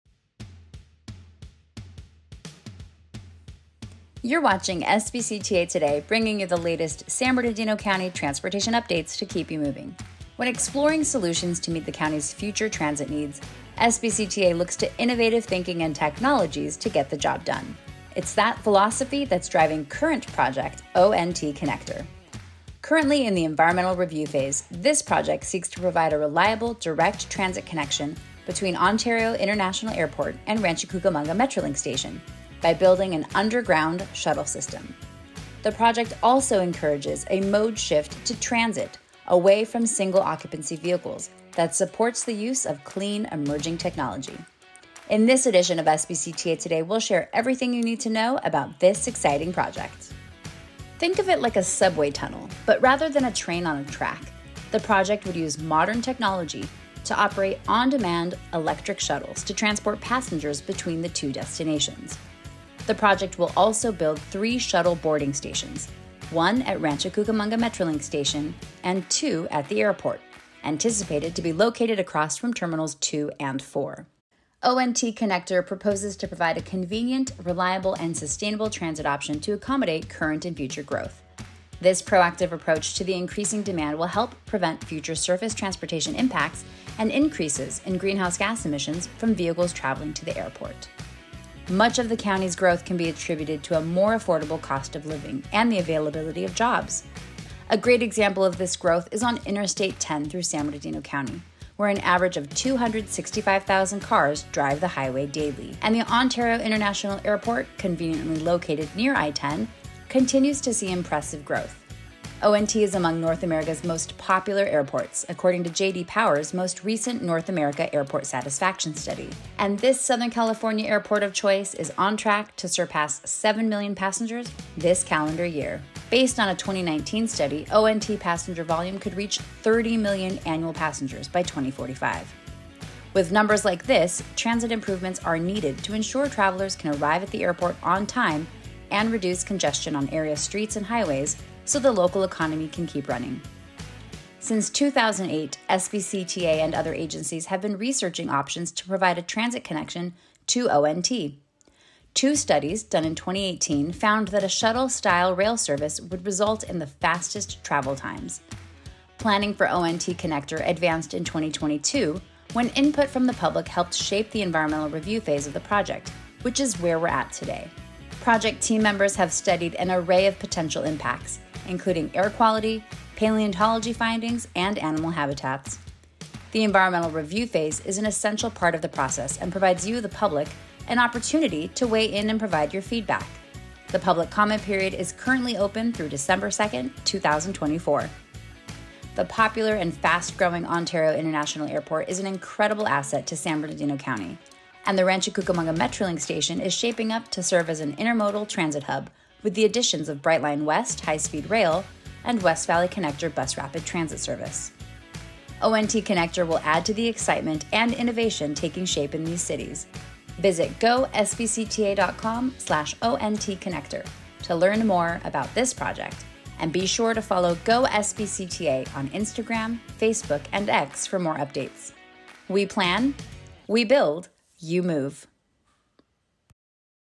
SBCTA Today: ONT Connector
SBCTA has partnered with Ontario International Airport (ONT) to develop forward-thinking transit solutions for current and future passengers while ensuring neighboring communities experience reduced congestion. This proposed ONT Connector project will create an underground transit connection between the Rancho Cucamonga Metrolink Station and ONT terminals. The project would seamlessly integrate into the Rancho Cucamonga Metrolink Station, which will also serve as the terminus for the privately-funded Brightline West electrified high-speed rail connection between Las Vegas and Southern California, as well as the zero-emission West Valley Connector bus rapid transit service coming in 2026.

Learn more about this project at goSBCTA.com/ONTConnector.